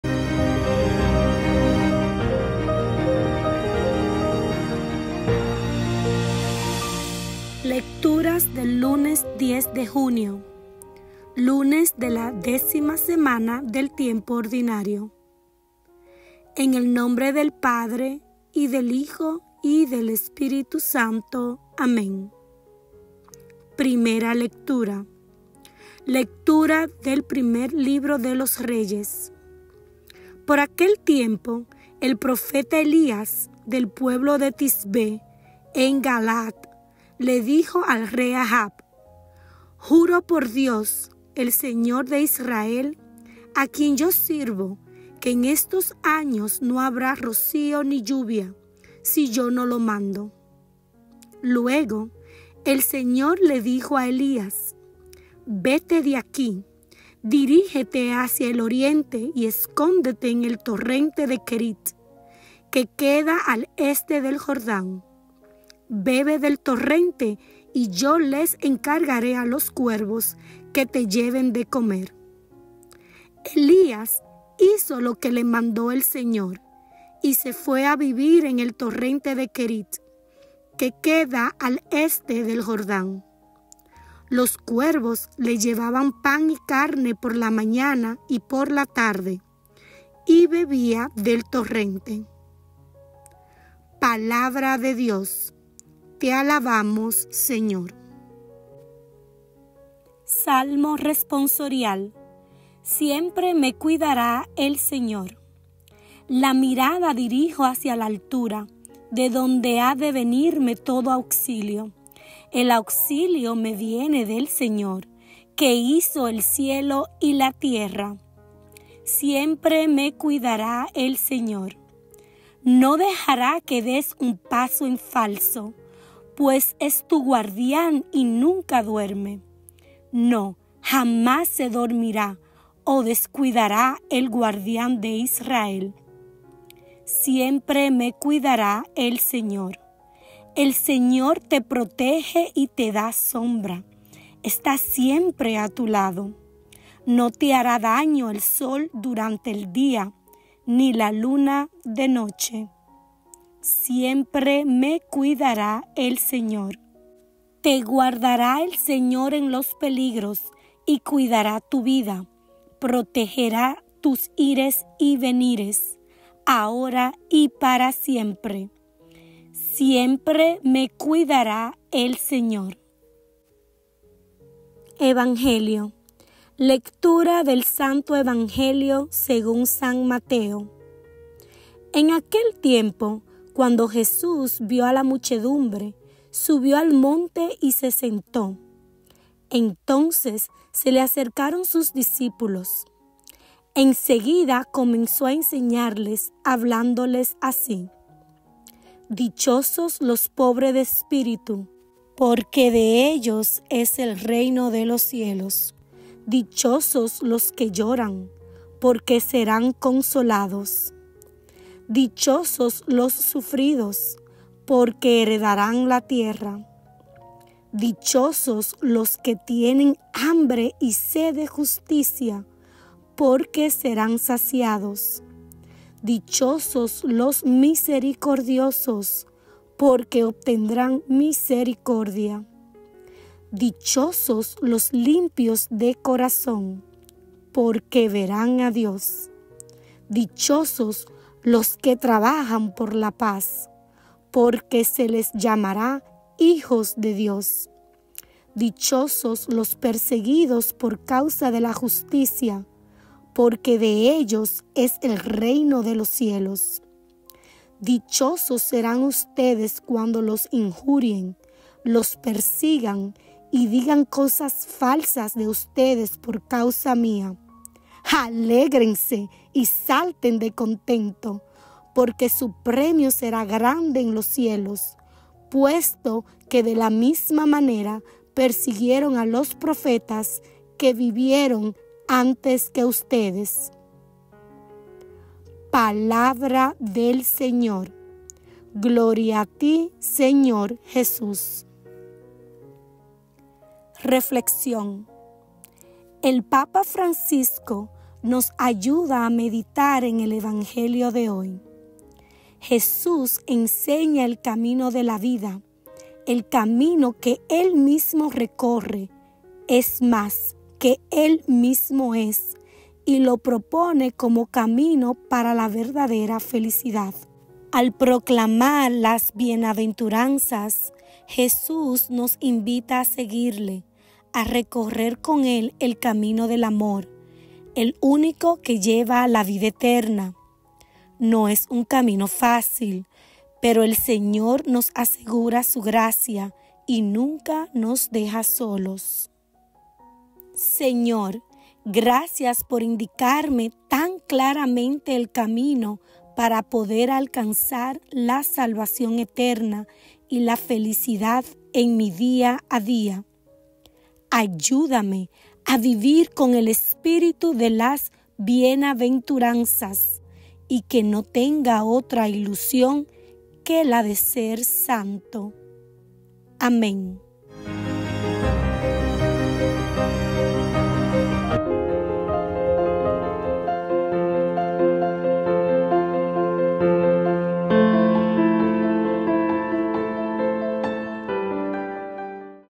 LECTURAS DEL DIA - 10 DE JUNIO
MMC presenta las Lecturas del dia con su correspondiente reflexión  como un recurso de crecimiento espiritual para todos,  esperamos que a través de este medio sigamos orando en familia y preservando nuestra vida de oración.

Fuentes:
Magnificat App y Evangeli.net